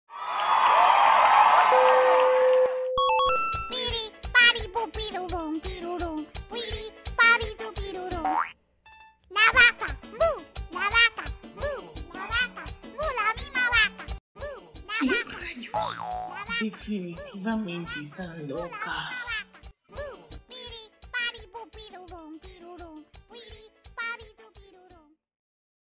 【SxF Minicomics】⭐ Incorret Quotes 01- 『Fandub parodia en Español 』
⭐Anime: SxF
⭐Audio/Video: Shary
⭐Fanarts: Shary
⭐Voces: Shary y Button

"Quienes te rompen el Timpano Pape (?)"
✪ Creditos/Credits: A Buttoneyes2 de Tiktok por su audio de la vaca xD
Ahora sí, LEA AQUÍ LA DESCRIPCIÓN! :
HELLOWWWWSES, JAU AURI YIUSES (?)!!! 1er IQ de SxF No soy ni seria, valee 🤣pero al oir la canción dije que esto es algo que Anya y Becky si cantarían. jajaja

-⭐Gracias a ustedes por esperar a que yo suba algo Q___Q **Cries in porky** !!!:D
CAMBIO Y FUERA XD